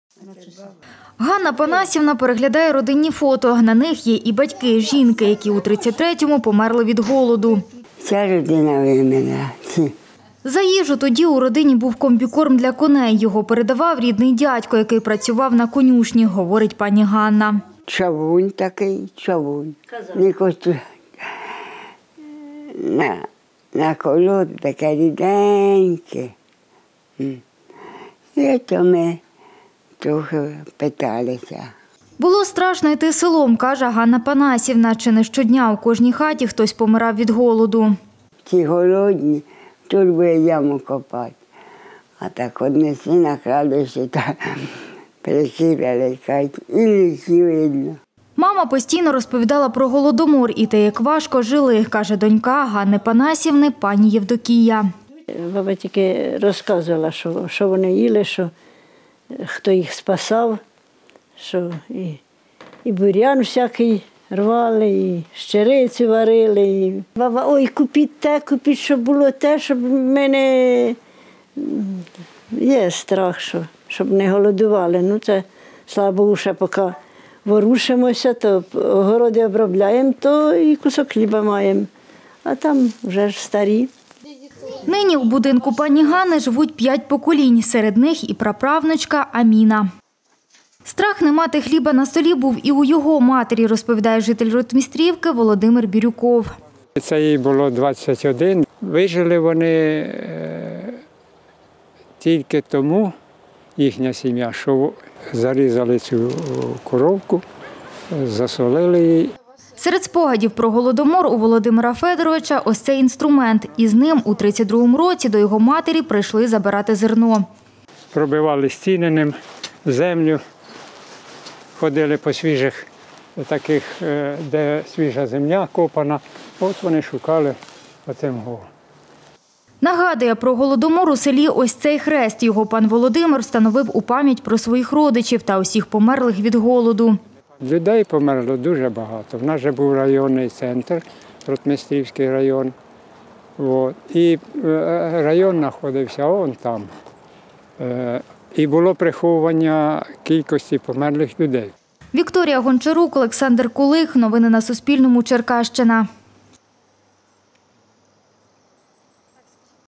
Хліб обов'язково має бути на столі
«Хліб обов'язково має бути на столі» – таке правило у родині жительки села Ротмістрівка, 99-річної Ганни Макоти. Жінка пережила два голодомори та війну. Ці часи називає жахливими. Саме тоді 11-річній дівчинці доводилося часто бути голодною та боротися за крихту хліба. А от її односельчанин Володимир Бірюков хоч голодомор і не бачив на власні очі, про ці події знає чимало із вуст своєї матері.